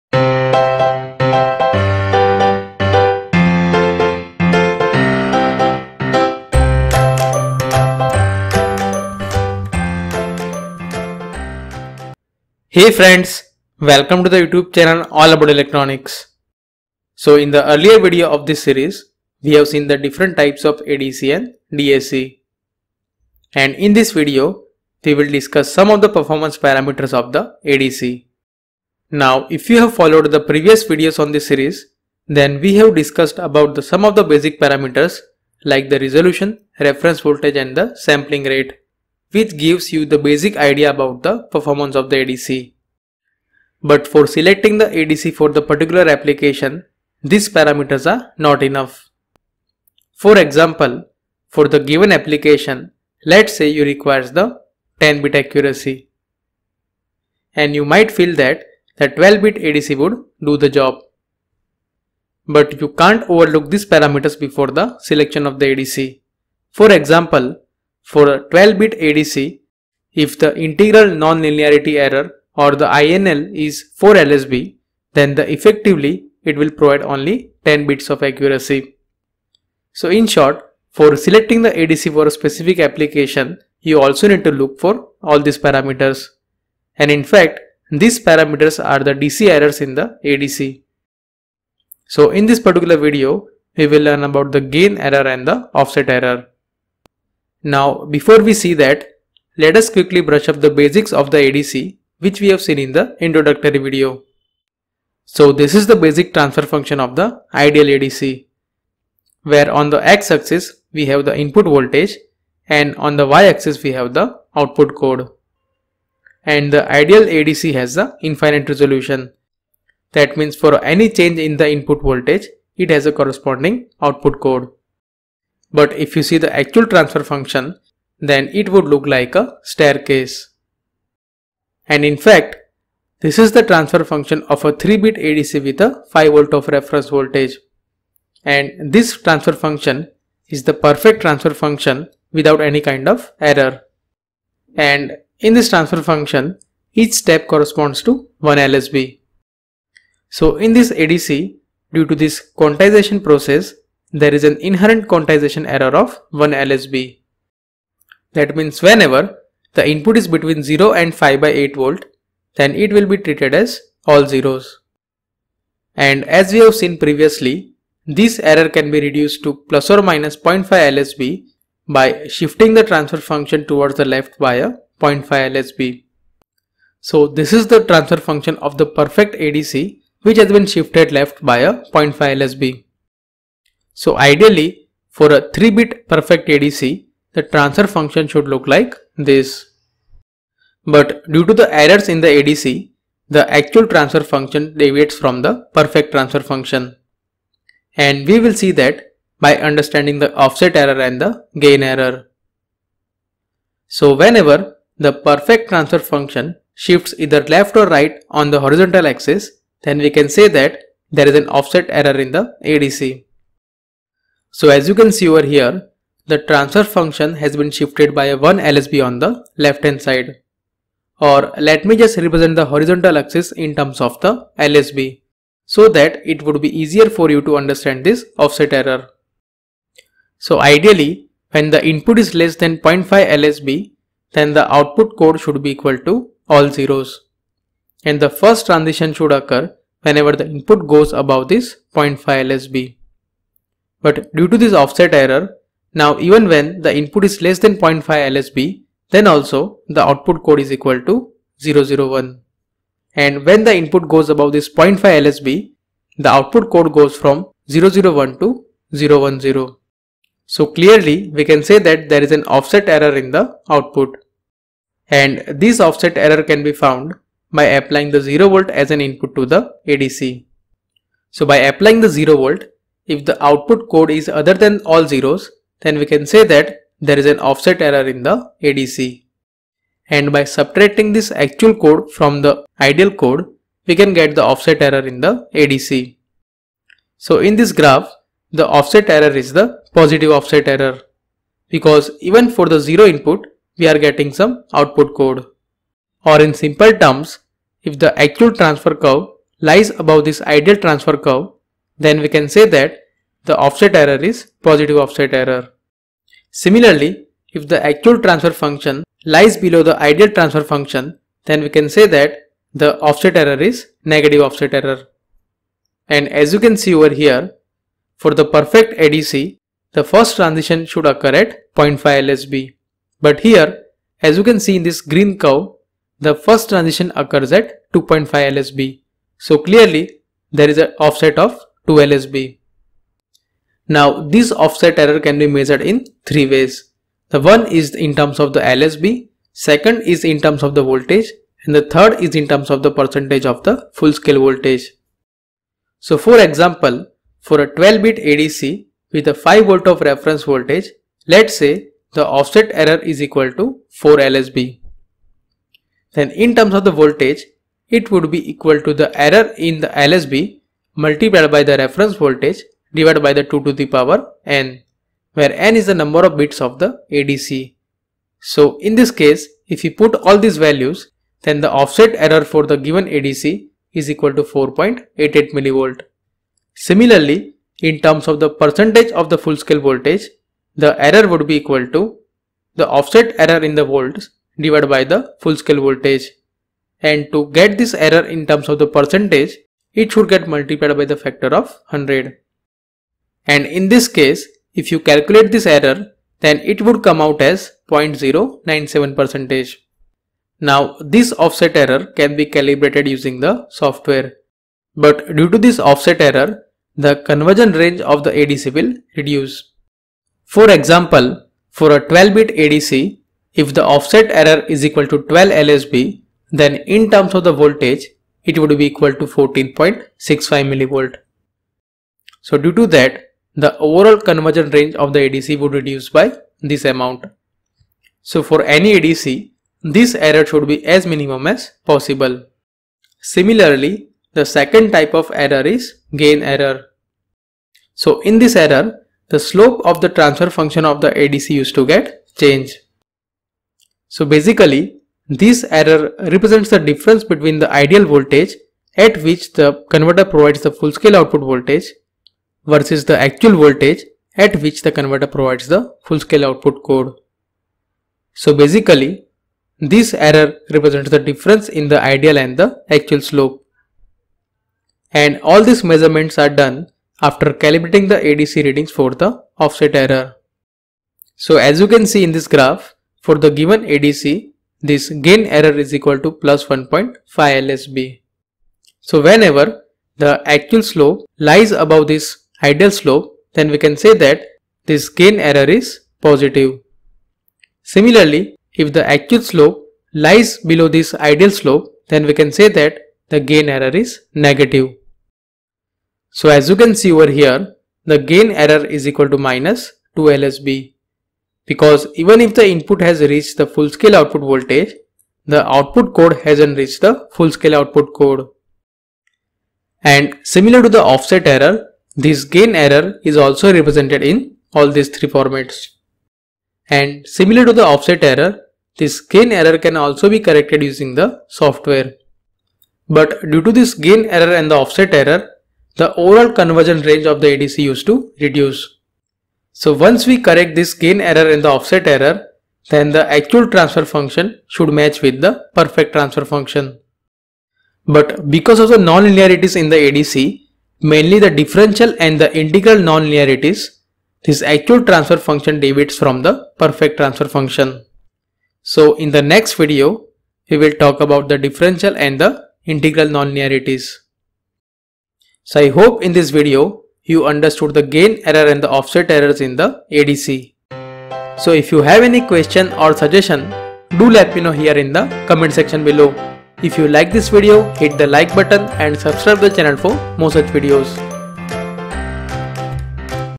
ADC : Offset Error and Gain Error Explained
In this video, the Offset Error and the Gain Error in the ADC has been explained.

By watching this video you will learn the following topics:

1:45 The Difference between Ideal, Perfect and the Actual Transfer Function of the ADC

3:23 ADC Offset Error

7:16 ADC Gain Error

For ADC and DAC there are many important parameters like

1) Resolution
2) Reference Voltage
3) Sampling Rate
4) Bandwidth
5) DC Errors
6) Signal to Noise Ratio
7) Effective Number of Bits (ENOB) etc.

Generally, the emphasis is given to only the Resolution  Reference Voltage and a Sampling rate of the ADC.
But other parameters are also very important for selecting the ADC for the specific application.
If other parameters are overlooked then one might not get the desired output using the ADC.

In this video, some of the DC errors in the ADC like Offset Error and the Gain Error has been explained.

This video will be helpful to everyone in understanding the Offset and the Gain Error in the ADC.